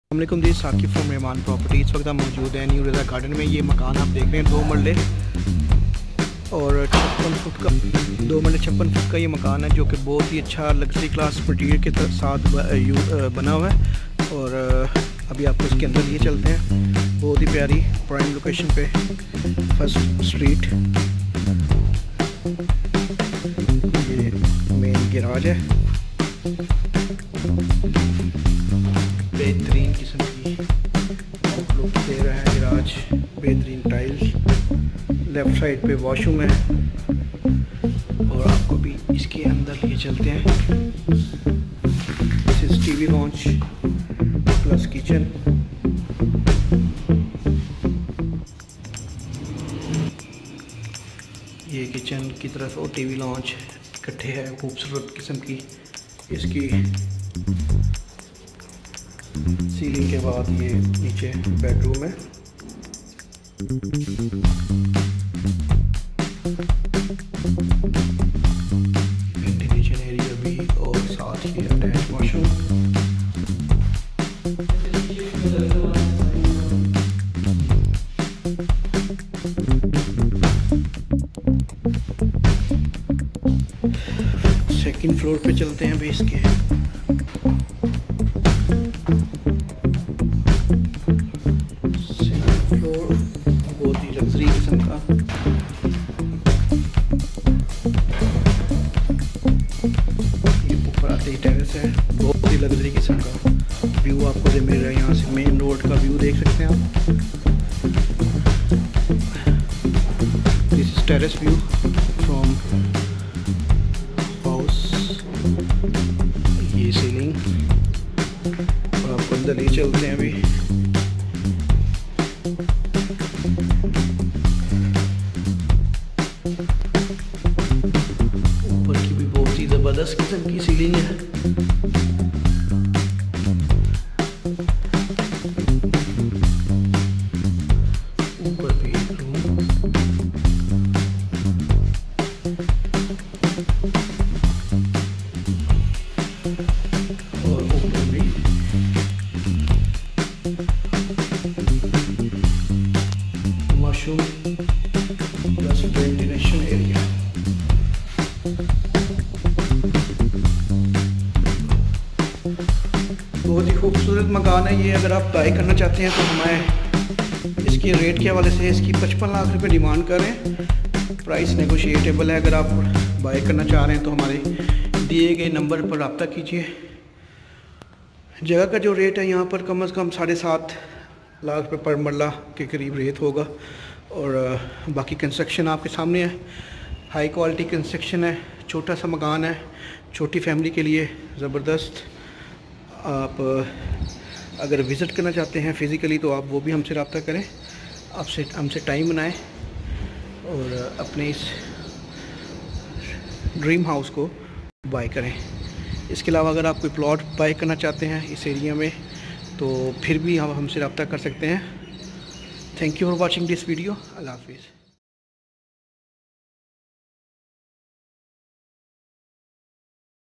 New Raza garden sargodha house for sale| 2.68 ft house
House for sale| in new raza garden sargodha
Price 50 lak demand
Sweet water underground electricity
Very near to city
3 rooms 3 washroom car garage
Luxury house
Price negotiate possible